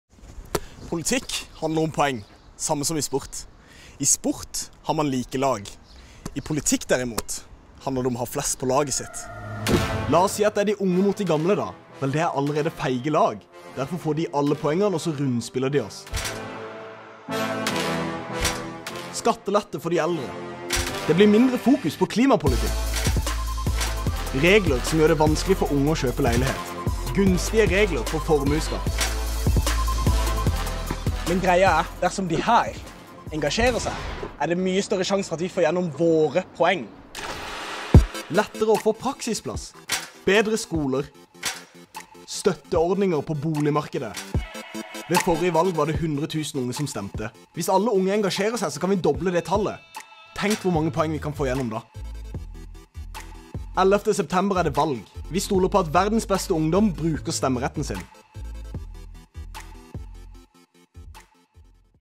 Skal vi la denne gjengen styre Norge? Unge stemmer 2017
Politikk handler om poeng. Samme som i sport, men i sport har man like lag. I politikk derimot, handler det om å ha flest på laget sitt.

Denne filmen er en del av kampanjen Unge stemmer, laget av Landsrådet for Norges barne- og ungdomsorganisasjoner (LNU) med støtte fra Valgdirektoratet.